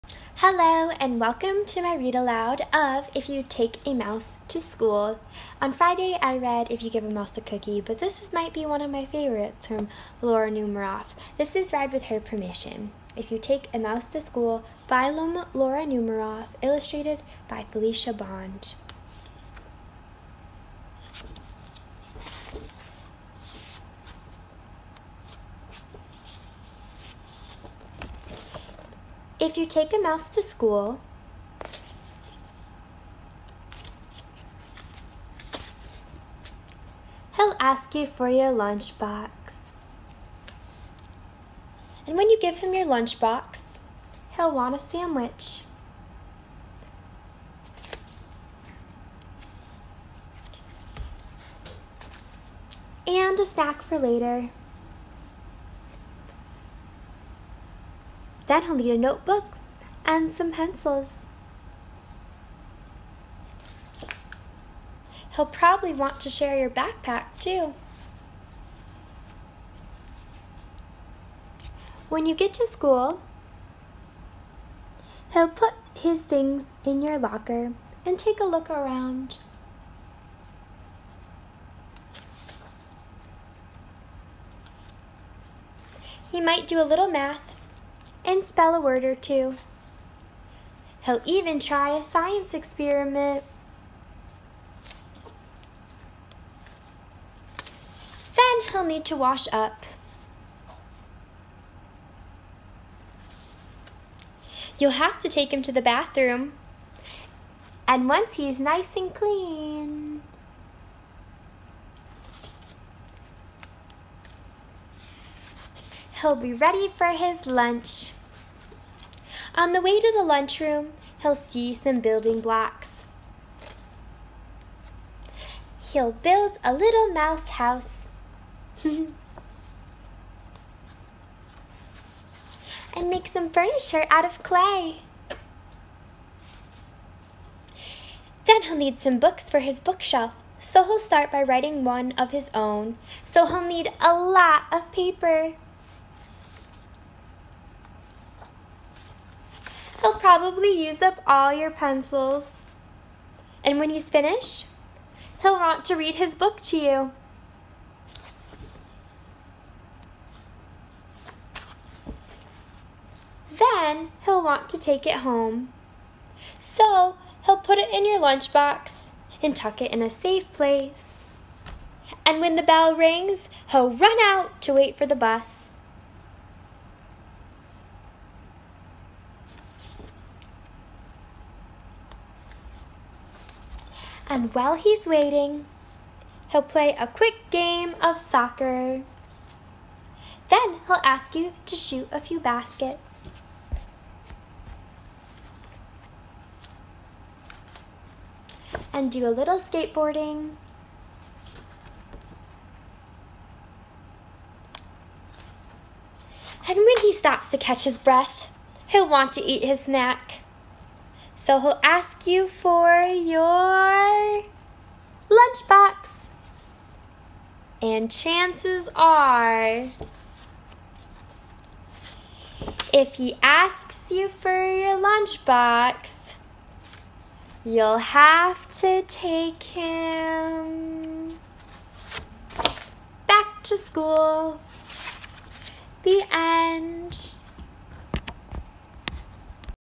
Tori Story: “If You Take a Mouse to School” by Laura Numeroff Illustrated by Felicia Bond
Read with Laura Numeroff’s permission